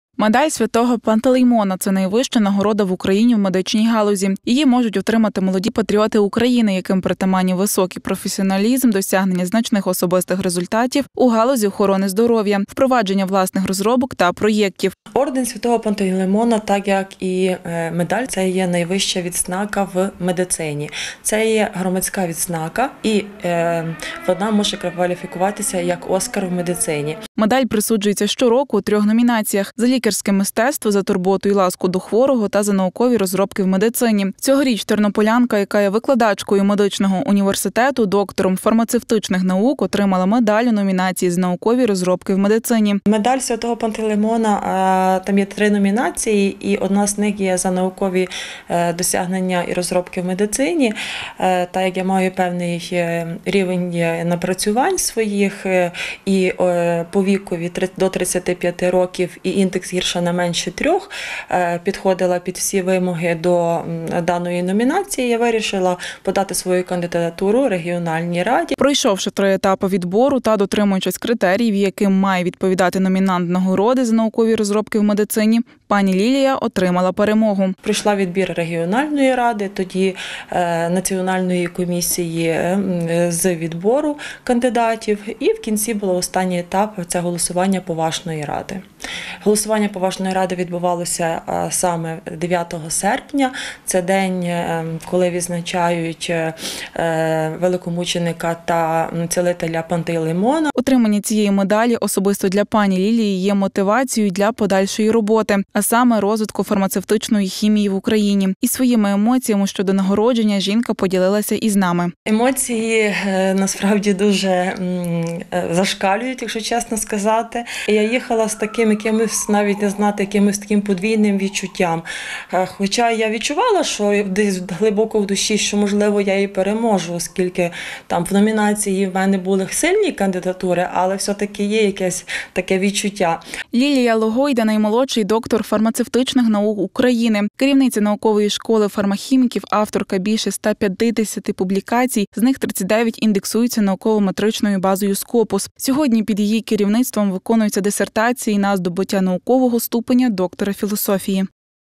Викладачка тернопільського вишу нагороджена медаллю Святого Пантелеймона
За відмінні результати в науково-дослідницькій діяльності та досягнення у сфері медицини, викладачку Тернопільського національного медичного університету нагородили медаллю Святого Пантелеймона.